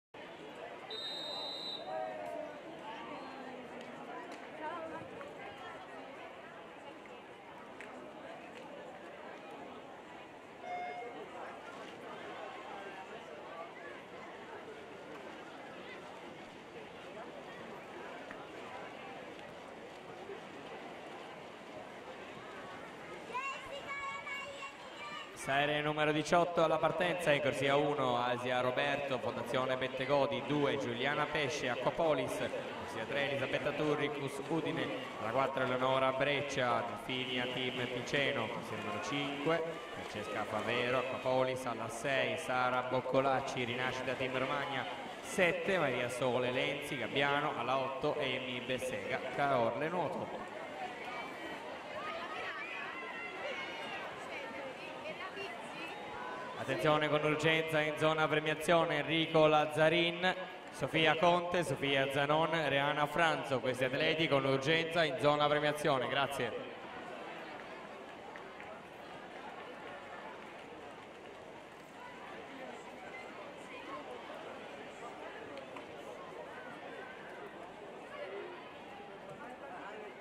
100 Stile Libero Ass. Femminile Agoniste (Serie 19) - 5 Treviso Swim Cup
Atleti partecipanti:
in corsia 1 SARTORATO GIADA (2006) della soc. HYDROS iscritta con 01:05.89
in corsia 2 BERTOLINI VIVIANA (2003) della soc. KUMA iscritta con 01:05.76
in corsia 3 SEGONI ANNA (2004) della soc. ASD ARCA NUOTO iscritta con 01:05.72
in corsia 4 RECCHIONI BEATRICE (2006) della soc. CIRCOLO NUOTO UISP iscritta con 01:05.66
in corsia 5 MENEGALDO EMMA (2004) della soc. HYDROS iscritta con 01:05.69
in corsia 6 BALICA DARIA VALENTINA (2006) della soc. CENTRO NUOTO CITTADELLA iscritta con 01:05.73
in corsia 7 BIANCHIN GEMMA (2006) della soc. HYDROS iscritta con 01:05.86
in corsia 8 MASAT VANESSA (2005) della soc. PARCO LIVENZA iscritta con 01:06.01